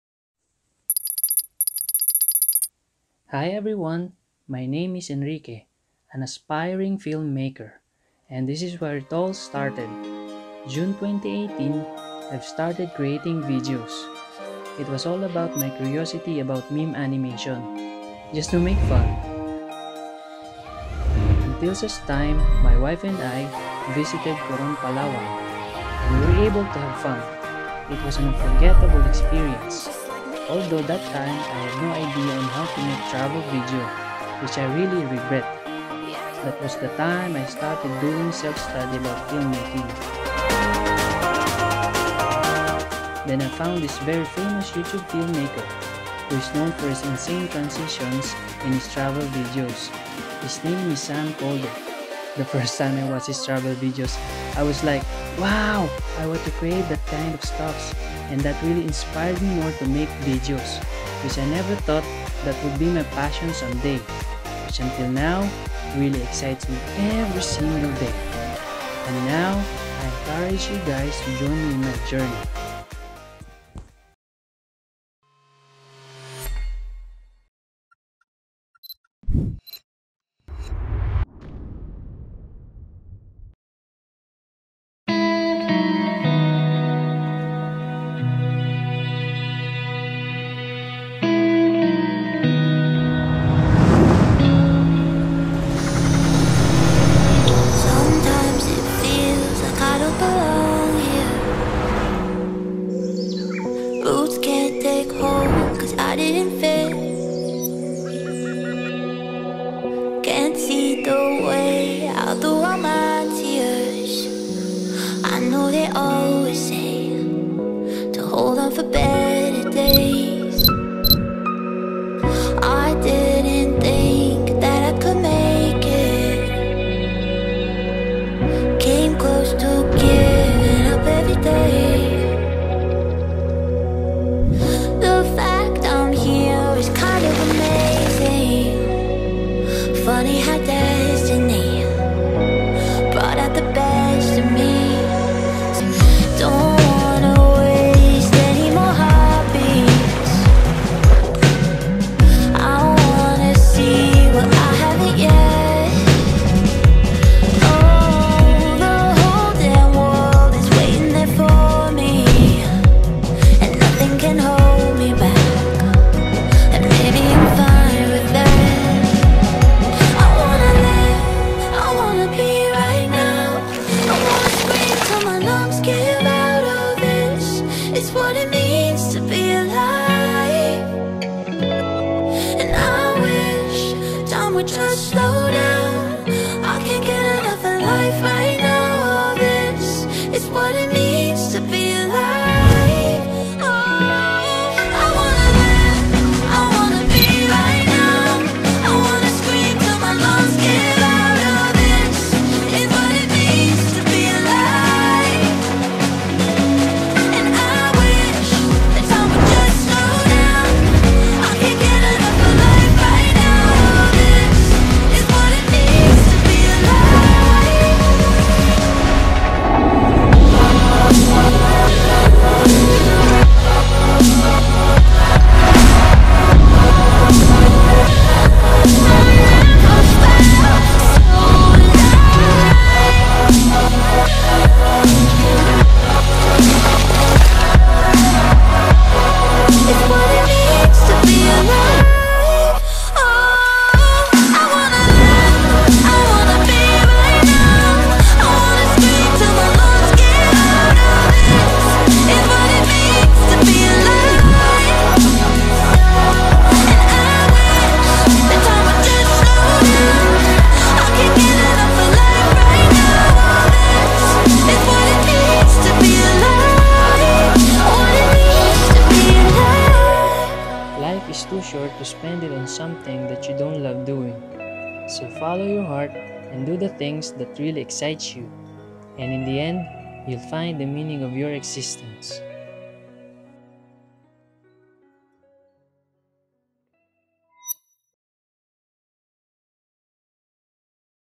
HOW I BECAME A FILM MAKER - Gen Tinio Nueva Ecija Travel Video
Never in my life i thought that i would be captivated by a camera until suddenly, i felt and fell in love with it out of nowhere, then i became a Film Maker.

Its been a month now since i started creating this video and its weird that i never experienced even a bit of fatigue. I put my heart and soul into this video. I hope you guys enjoy and be inspired.

If you guys like more of this video and want to support me, please share and make sure to subscribe to this channel and let me know your thoughts on this video.